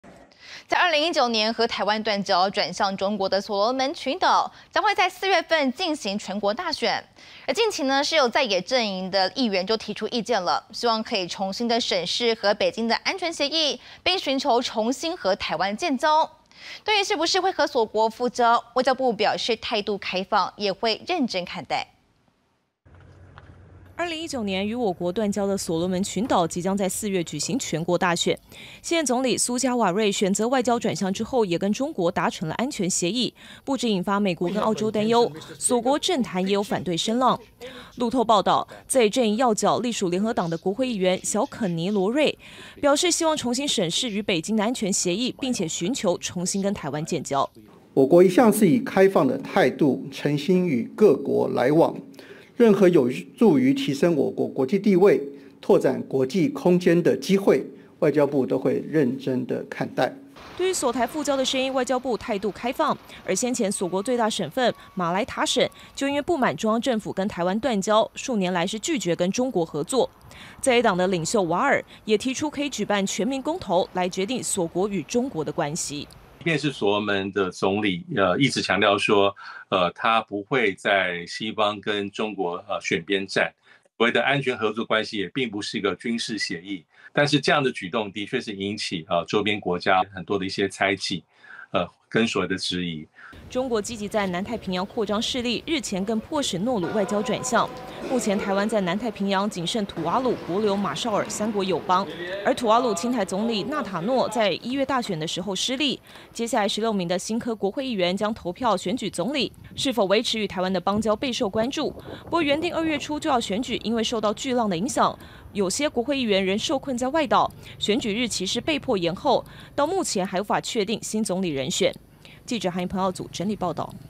索羅門群島在野黨拋復交意願 外交部：態度開放、認真看待｜20240215 公視晚間新聞
2019年選擇與台灣斷交轉向中國的索羅門群島，即將在4月舉行全國大選，近期在野陣營議員提出意見，希望重新審視與北京的安全協議，並尋求重新和台灣建交。對於是否和索國復交，外交部表示態度開放，也會認真看待。